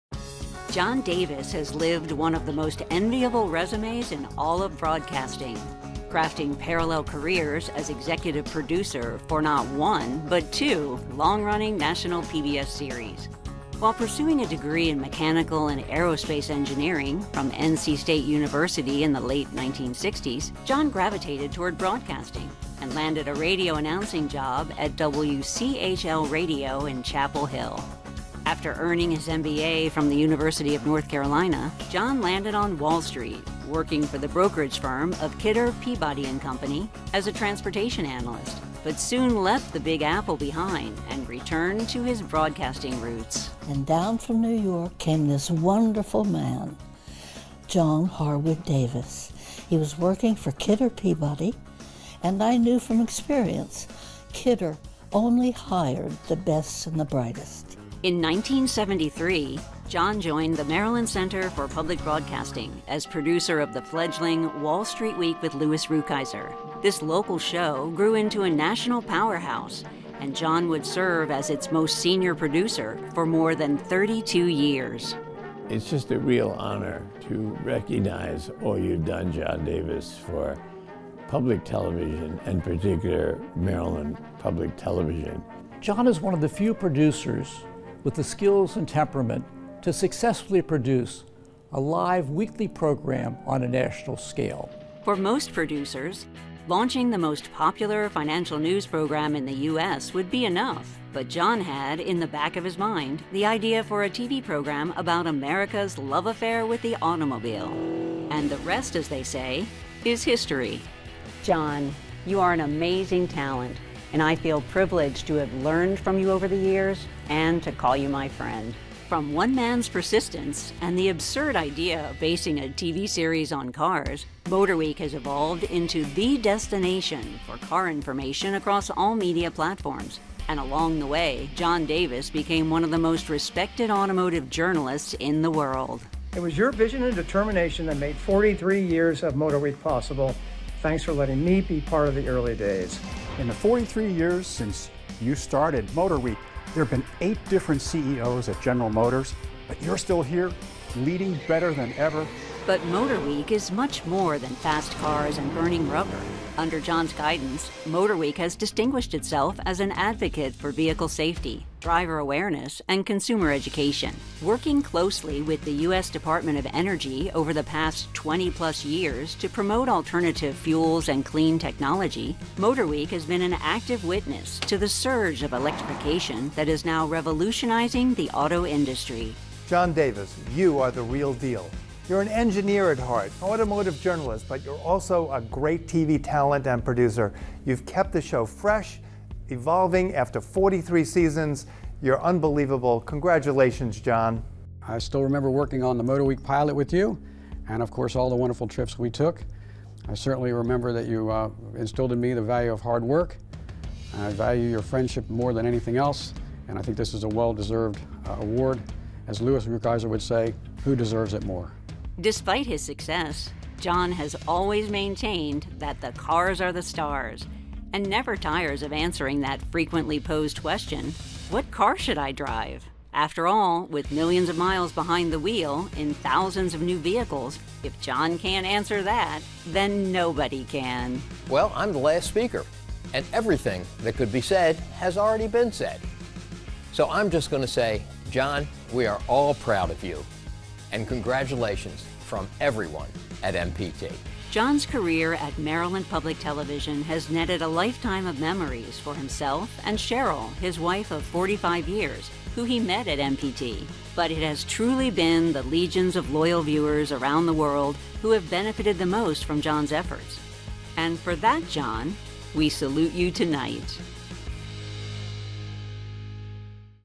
MotorWeek's John Davis is Honored for 50 years in Television!
We celebrate the illustrious career of John Davis as he is inducted into the prestigious Gold Circle of the Capital Emmys chapter of the National Academy of Television Arts and Sciences (NATAS-NCCB).

The Gold Circle honors individuals who have made significant contributions to the broadcast or cable industry for 50 years or more.

Congratulations on 50 years in television, John!